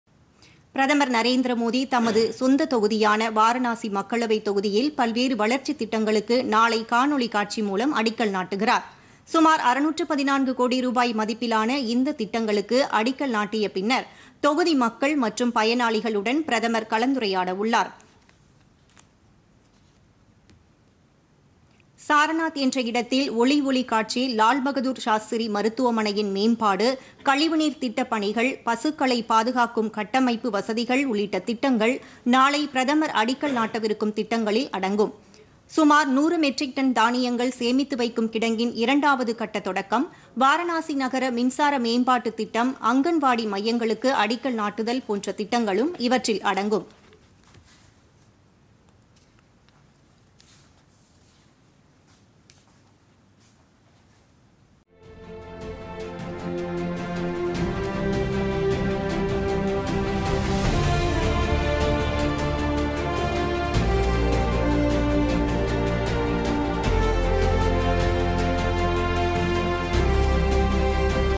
வாரணாசியில் வளர்ச்சித் திட்டப் பணிகள்
வாரணாசியில் 614 கோடி ரூபாய் மதிப்பிலான  வளர்ச்சித்  திட்டப் பணிகள் - காணொலி காட்சி மூலம் பிரதமர் நரேந்திர மோதி நாளை தொடங்கி வைக்கிறார்.